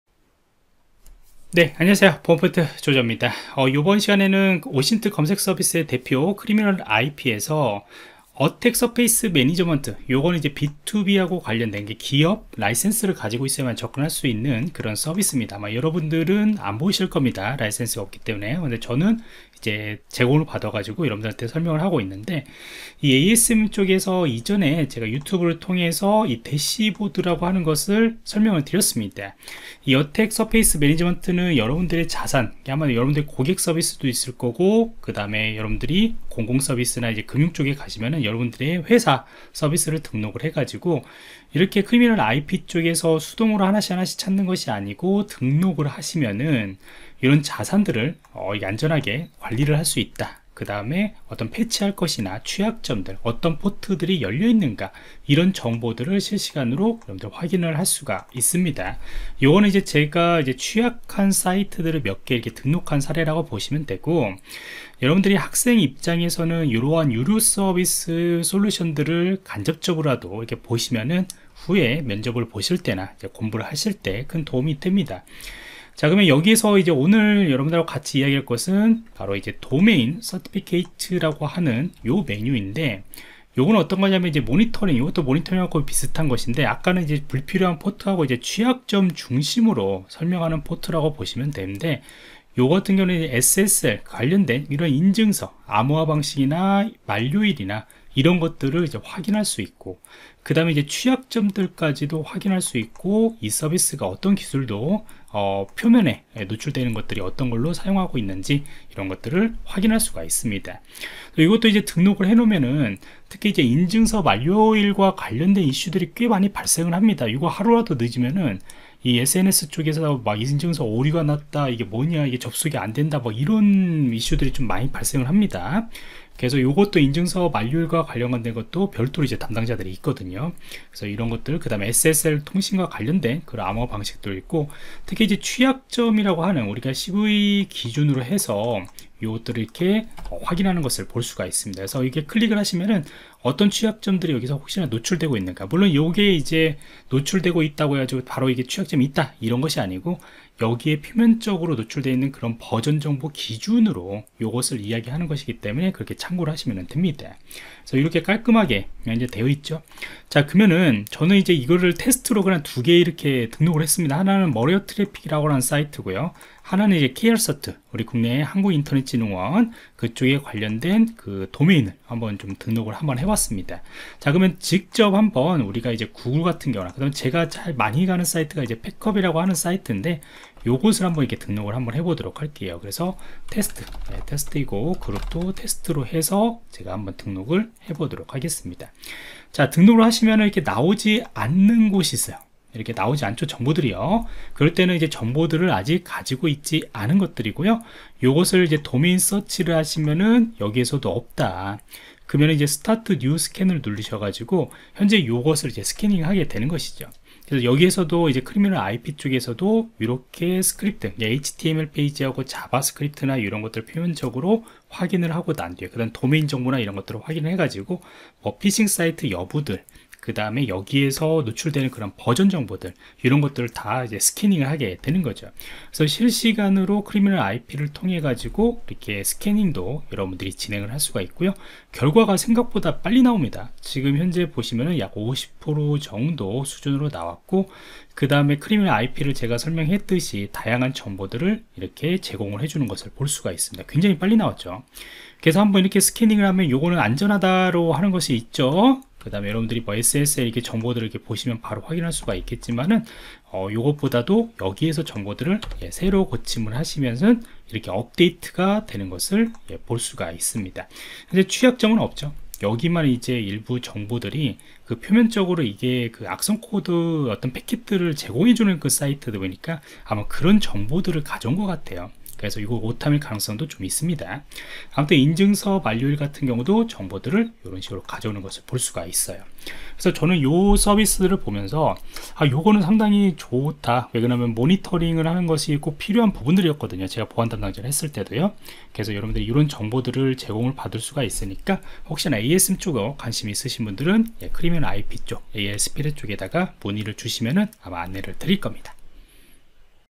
Criminal IP ASM의 인증서 도메인 관리 기능 소개
보안프로젝트에서 ) 제공하는 IT 강의입니다. 강의 주제는 "Criminal IP ASM의 인증서 도메인 관리 기능 소개 " 입니다.